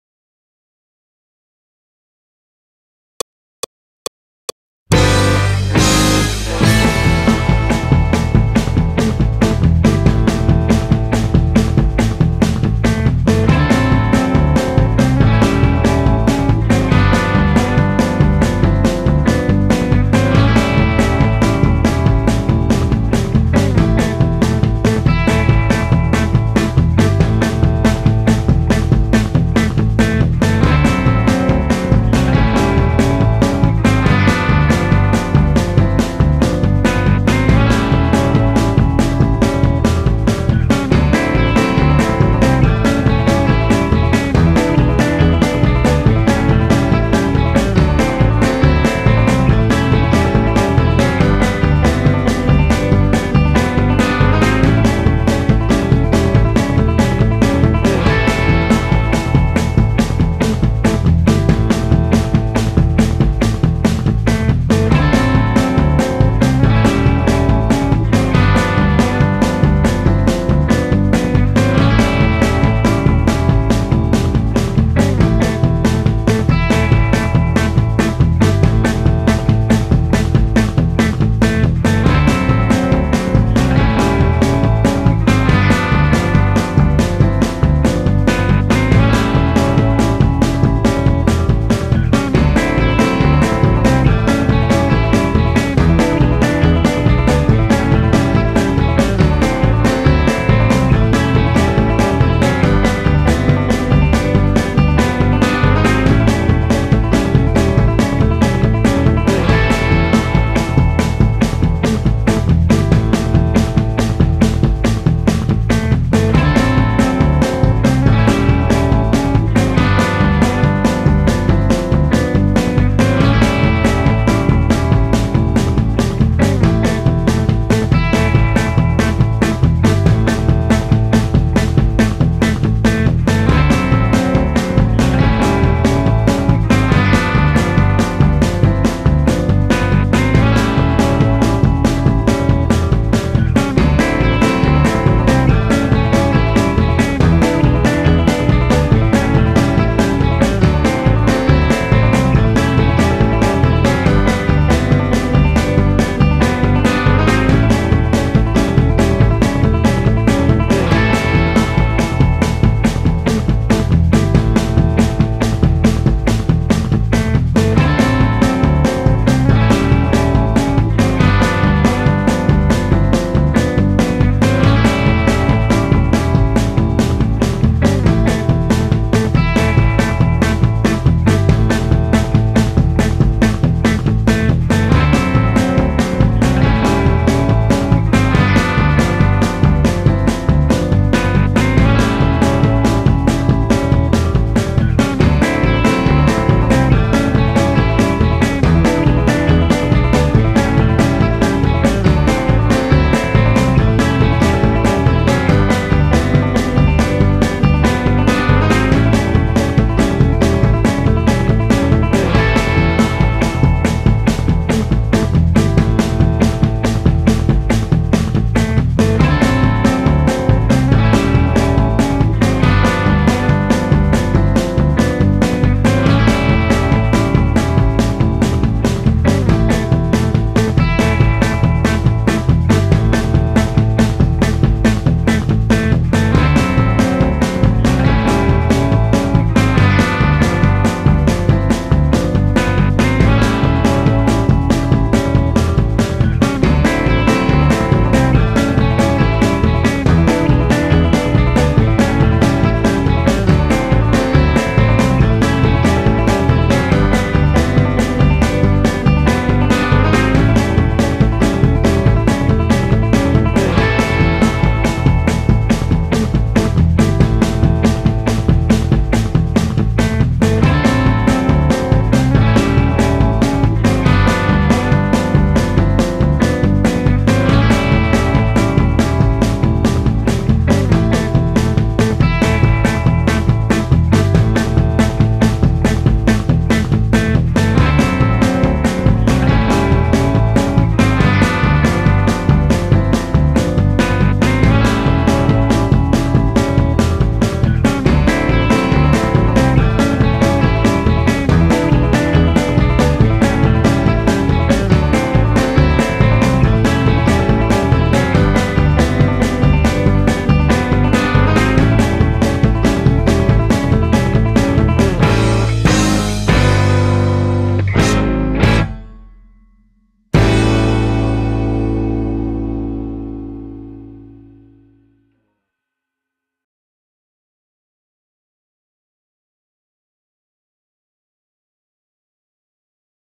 Country Backing Track - Key of A - 140 BPM - Mixolydian
For Weekly licks in your inbox pick up a subscription
For our first backing track we're going a little country! All licks from the "Open Strings" series and the "Much Less Impossible" series will work over this track! If you'd like to Download this track you can get it